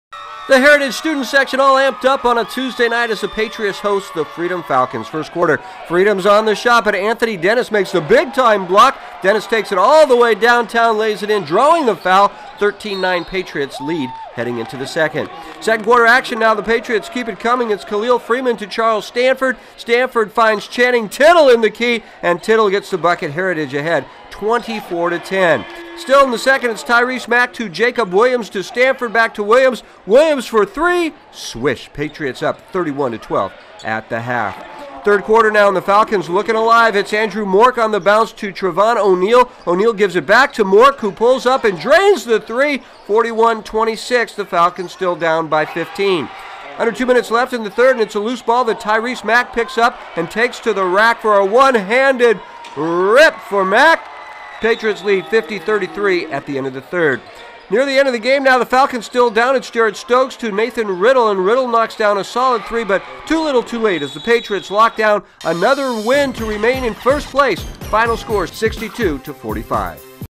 Freedom at Heritage BH 012917
IT’S THE HERITAGE STUDENT SECTION ALL AMPED UP ON A TUESDAY NIGHT, AS THE PATRIOTS HOST THE FALCONS FROM FREEDOM.
FIRST QUARTER, FREEDOM’S ON THE SHOT, BUT ANTHONY DENNIS MAKES THE BIG TIME BLOCK, DENNIS TAKES IT ALL THE WAY DOWNTOWN, LAYS IT IN, AND DRAWS THE FOUL, 13-9 PATRIOTS LEAD HEADING INTO THE 2ND.
2ND QUARTER, PATRIOTS KEEP IT COMING, IT’S KALIL FREEMAN TO CHARLES STANFORD, STANFORD FINDS CHANNING TIDDLE IN THE KEY, AND TIDDLE GETS THE BUCKET, HERITAGE AHEAD 24-10.
STILL THE 2ND, IT’S TYRESE MACK TO JACOB WILLIAMS TO STANFORD, BACK TO WILLIAMS, WILLIAMS FOR THREE, SWISHHH, PATRIOTS UP 31-12 AT THE HALF.
3RD QUARTER, FALCONS LOOKING ALIVE, IT’S ANDREW MORK ON THE BOUNCE TO TREVON O’NEAL , O’NEAL GIVES IT BACK TO MORK WHO PULLS UP AND DRAINS THE 3, 41-26 FALCONS STILL DOWN.
UNDER 2 LEFT IN THE 3RD, AND IT’S A LOOSE BALL THAT TYRESE MACK PICKS UP AND TAKES TO THE HOOP, FOR A ONE-HANDED DUNK! PATRIOTS LEAD 50-33 BY THE END OF THE 3RD.
NEAR THE END OF THE 4TH, FALCONS STILL DOWN, IT’S JARED STOKES TO NATHAN RIDL, RIDL KNOCKS DOWN A SOLID 3, BUT IT’S TOO LITTLE TOO LATE
AND THE PATRIOTS LOCK DOWN ANOTHER WIN, FINAL SCORE 62-45.